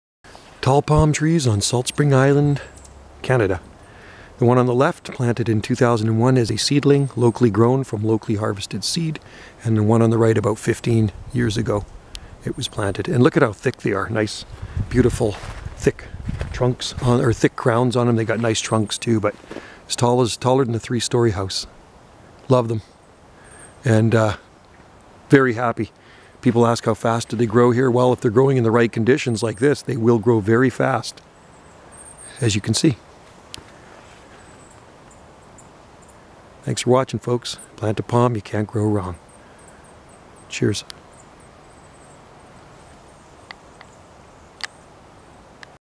Tall Windmill Palm trees on Salt Spring Island, Canada
Trachycarpus fortunei palms grow tall on the Gulf Islands